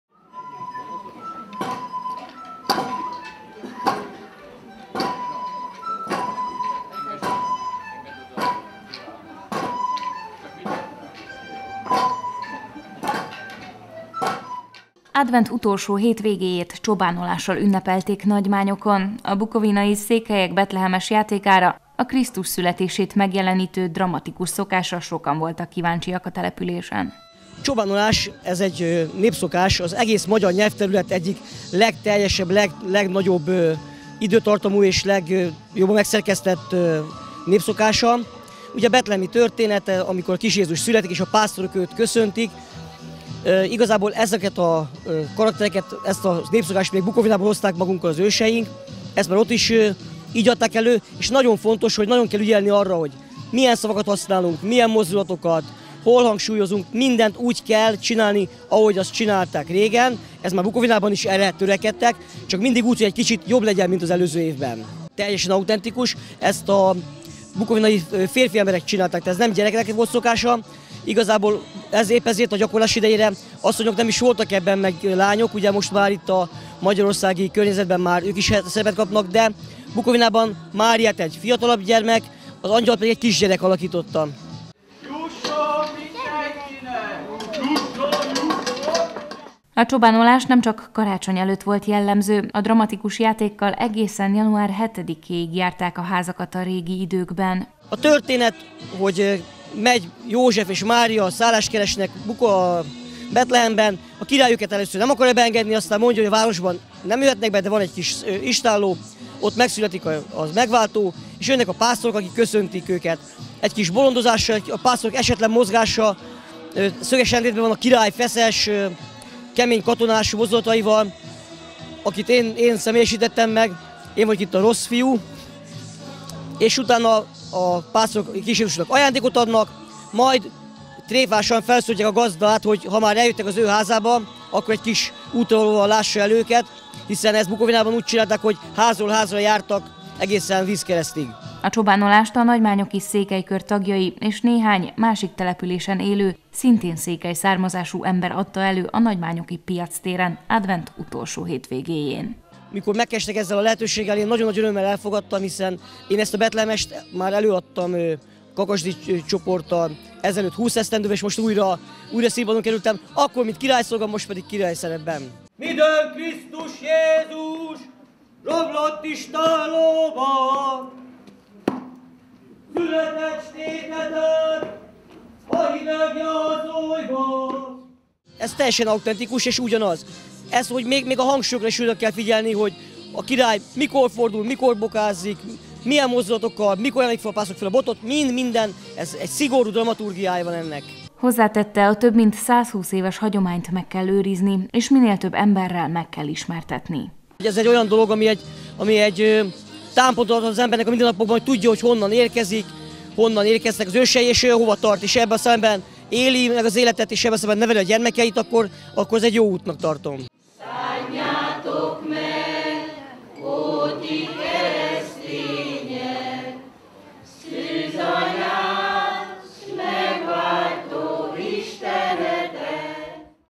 A Nagymányoki Székelyek is tovább vitték a csobánolás hagyományát
Jézus születésének történetét adták elő a nagymányoki székelykör tagjai a településen, advent utolsó hétvégéjén. Mint ahogy azt az előzőekben hallottuk a csobánolás egy olyan hagyomány, melyben a szövegen és a mozdulatokon sem változtattak ez idő alatt. Ezt szeretnék minél több emberrel megismertetni.